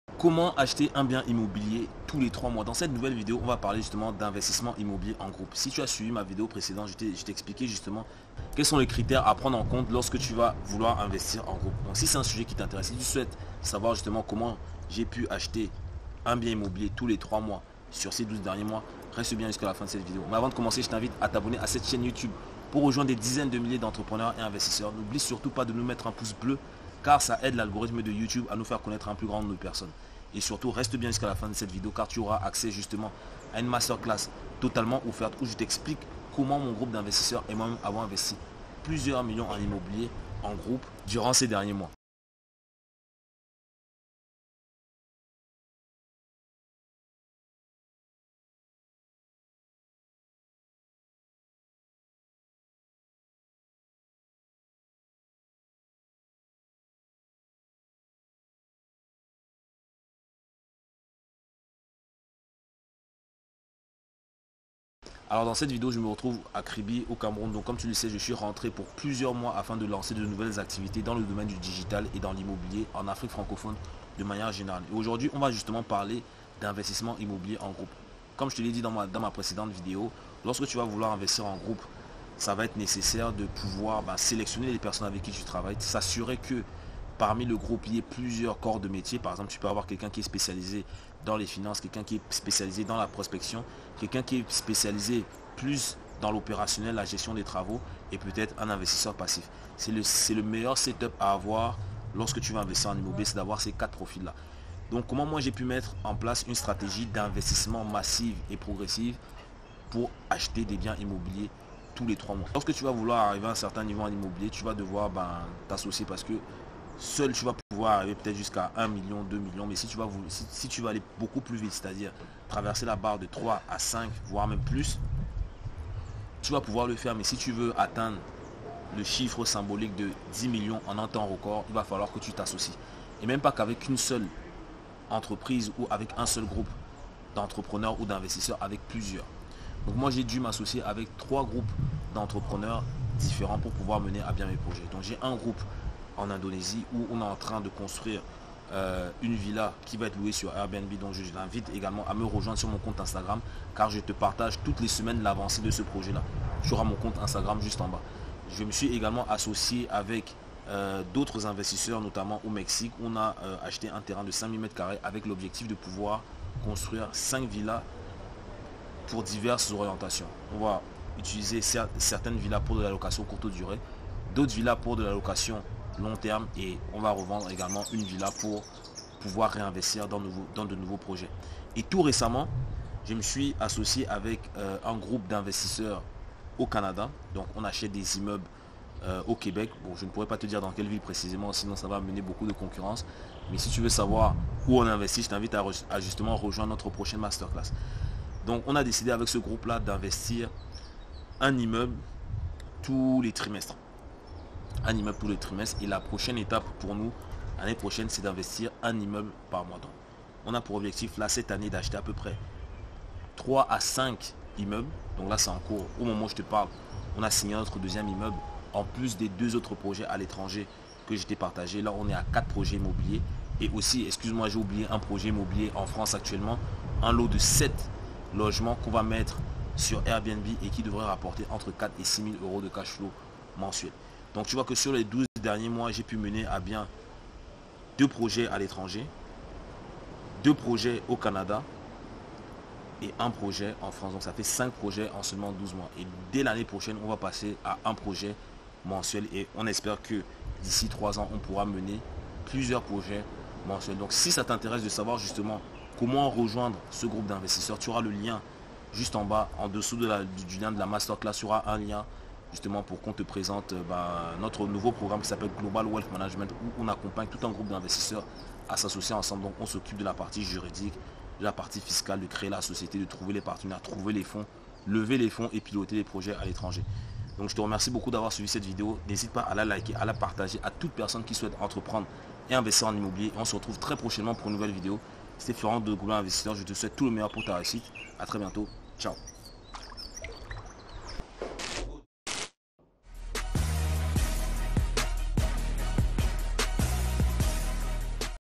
Comment j'Achète Un Bien immobilier Tous Les 3 Mois
Dans cette nouvelle vidéo dans t'explique comment j'achète un bien immobilier tous les 3 mois

Avertissement